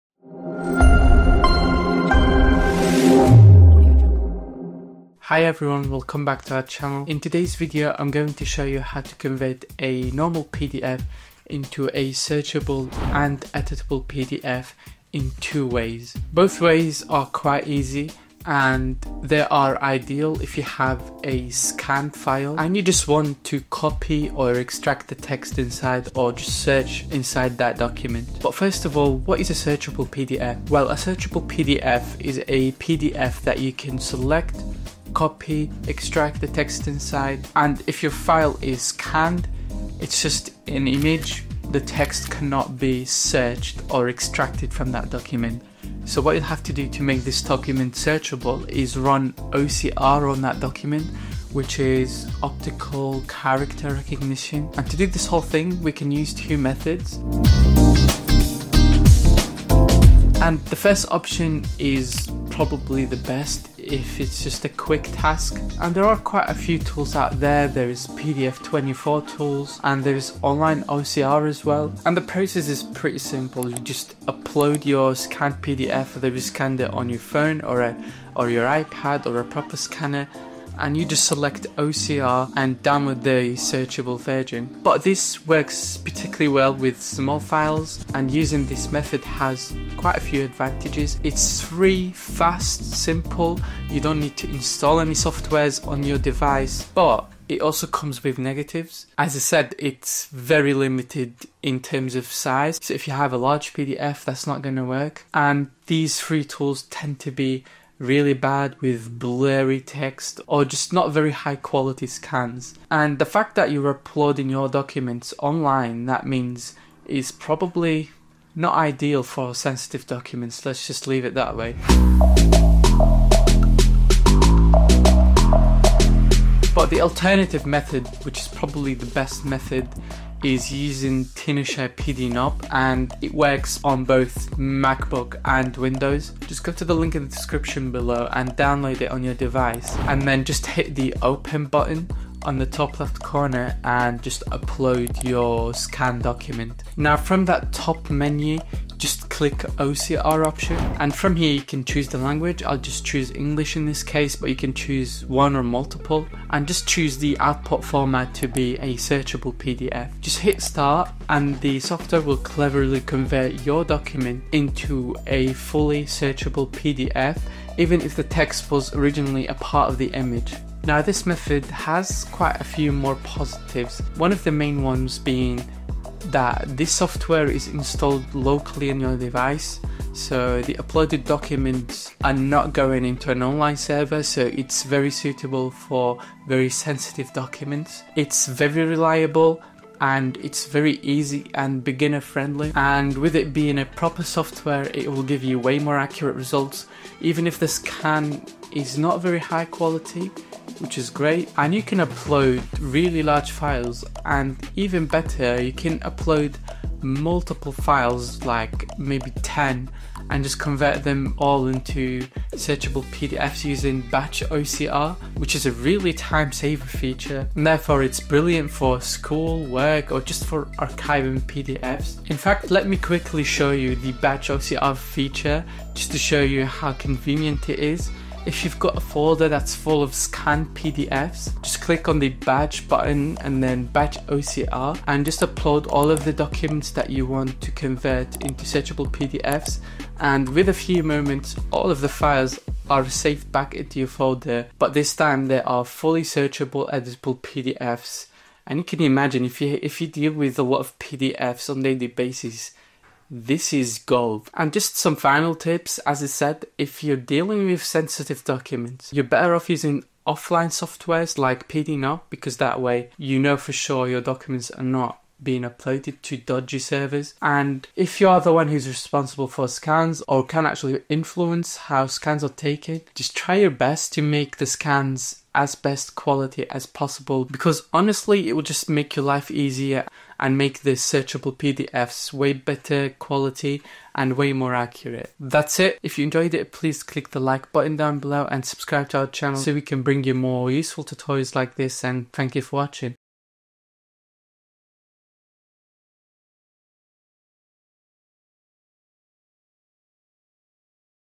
How to Convert PDF to Searchable PDF | 2 Easy Ways Including Free Online
Tenorshare PDNob Convert PDF to Searchable PDF. Convert, OCR PDF with Ease

In this video, you'll learn how to easily convert a scanned or image-based PDF into a fully searchable PDF — so you can find, select, and copy text effortlessly. We’ll show both a free online method for quick use and a more powerful offline solution using Tenorshare PDNob on Windows or Mac.

⏱ *TIMESTAMP*
00:00 Introduction – What is a Searchable PDF
00:42 Method 1: Convert PDF to Searchable PDF Online Free
02:05 Method 2: Convert PDF to Searchable PDF on Windows & Mac

04:14 Conclusion

💡 *How to Convert PDF to Searchable PDF Summary*
🔹Method 1: Convert PDF to Searchable PDF Online Free
- Visit a free online OCR tool
- Upload your scanned or image-based PDF.

🔹Method 2: Convert PDF to Searchable PDF on Windows & Mac
- Download and install Tenorshare PDNob on your Windows or Mac.
- Launch the program and go to the OCR section.
- Upload your scanned or image-based PDF file.
- Choose the output format as Searchable PDF.
- Click "Perfrom OCR" and wait for the process to finish.

How to extract text from scanned pdf to word